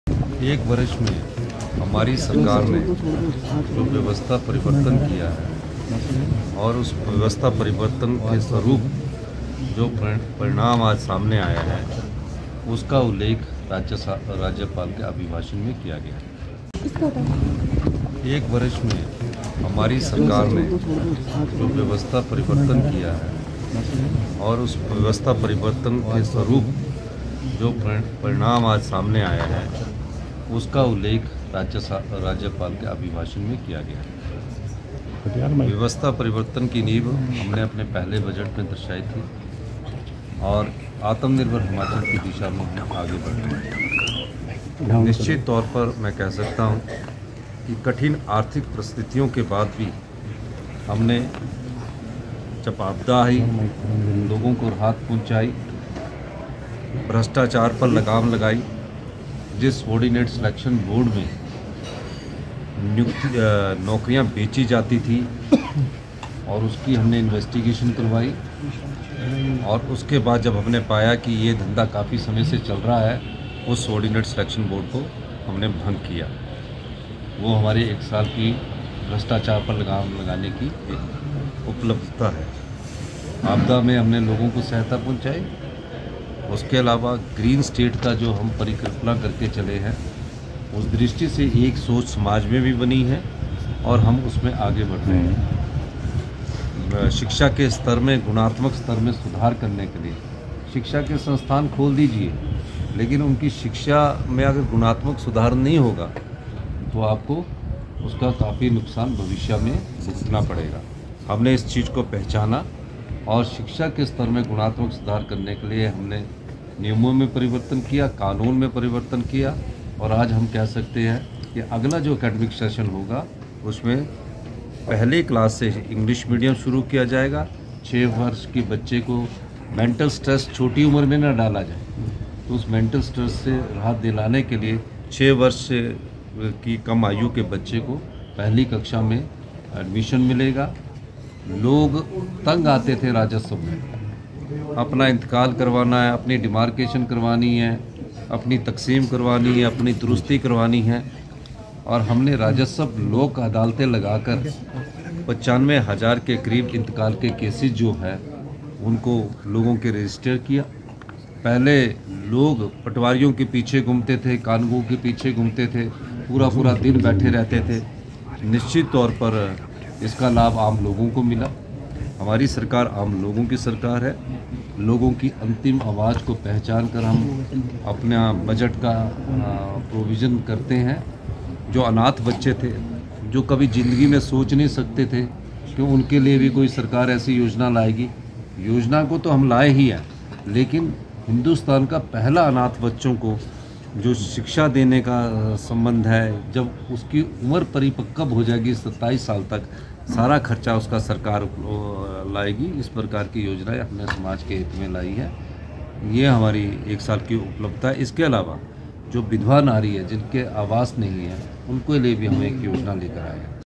CM Sukhu/government/ Governor
CM Sukhwinder Singh Sukhu said that the results of the system changes made by our government in one year have been mentioned by the Governor in his address. We are moving towards self-reliant Himachal. We curbed corruption. Made qualitative improvements in the level of education.